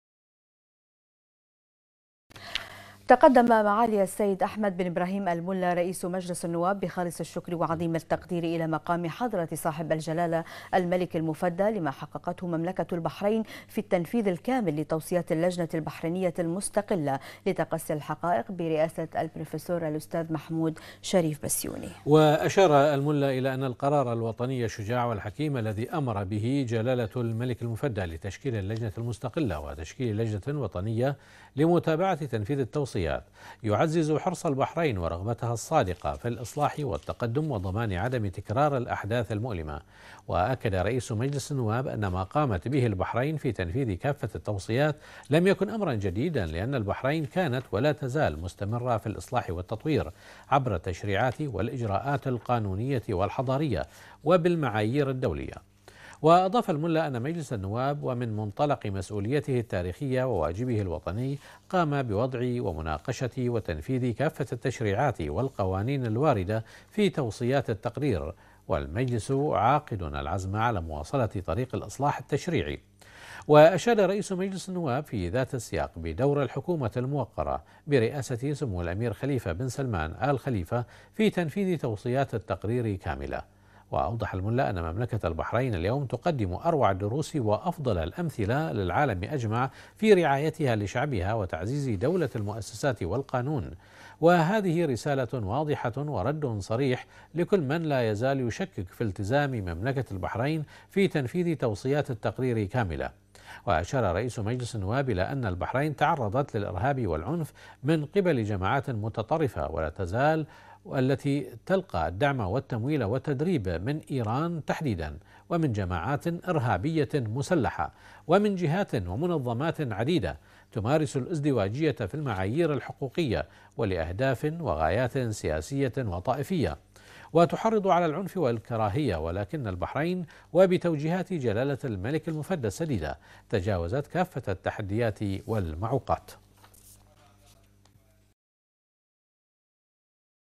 البحرين : تقدم معالي السيد أحمد بن إبراهيم الملا رئيس مجلس النواب بخالص الشكر وعظيم التقدير
تقدم معالي السيد أحمد بن إبراهيم الملا رئيس مجلس النواب بخالص الشكر وعظيم التقدير الى مقام حضرة صاحب الجلالة الملك المفدى لما حققته مملكة البحرين في التنفيذ الكامل لتوصيات للجنة البحرينية المستقلة لتقصي الحقائق